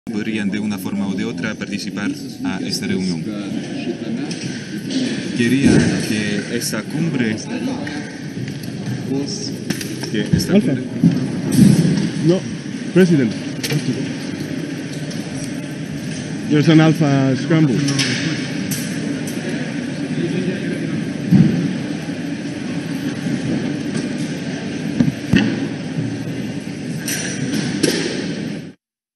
❗ Pedro Sánchez interrumpe una rueda de prensa en Lituania por una amenaza de la Fuerza Aérea Rusa
➤ Pedro Sánchez se ha visto obligado a interrumpir una rueda de prensa en Lituania por una amenaza real de la Fuerza Aérea Rusa